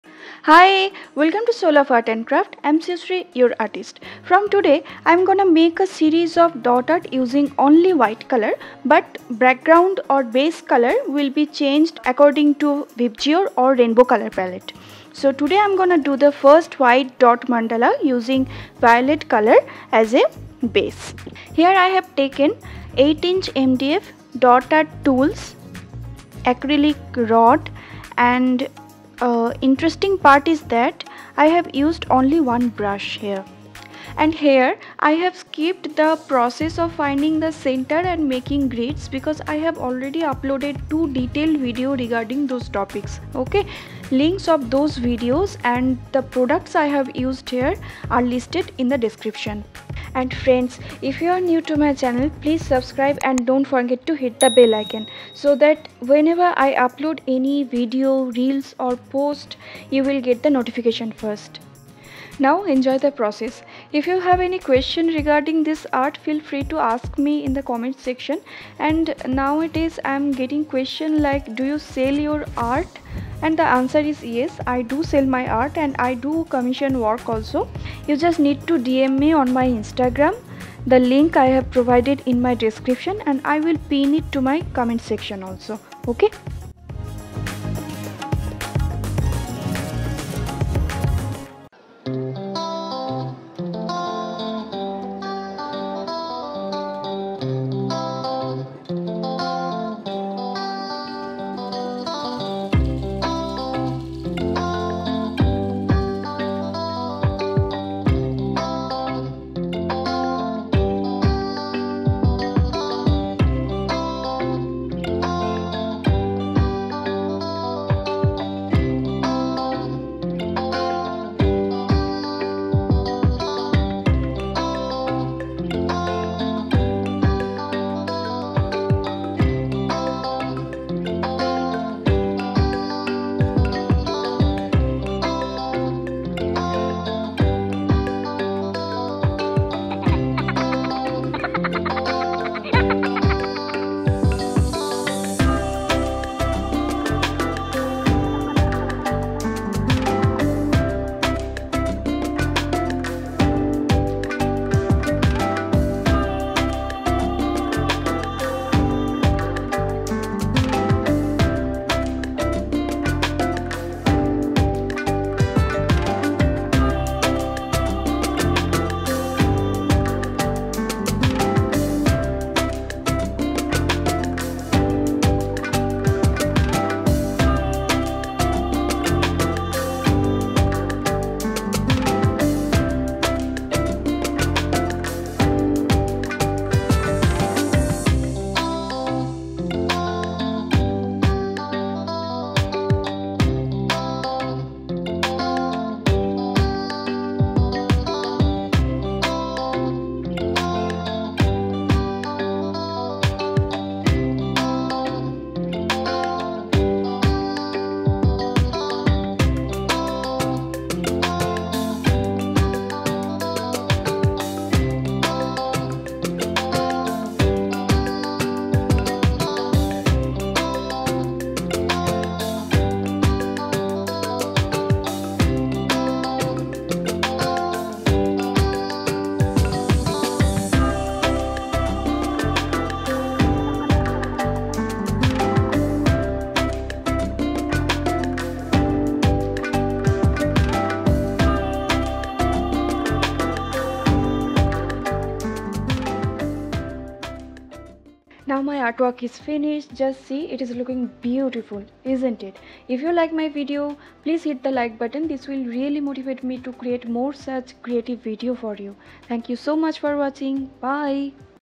Mesmerizing WHITE Dot Mandala Painting on VIOLET color tutorial
Mesmerizing WHITE Dot Mandala Painting on VIOLET color || Rainbow Background series Part 1

Materials I have used here: -
Mdf
Color
Violet and White
[I have used here mont marte white colour simple to thicken up the consistency ]
Pencil